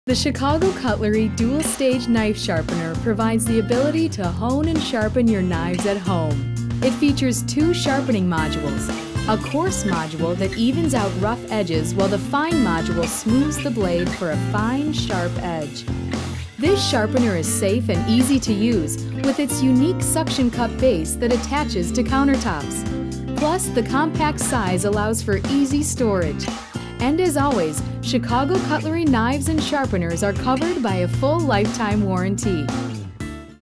Chicago Cutlery® Dual Sharpener
The Dual Stage Knife Sharpener provides the ability to hone and sharpen your knives at home!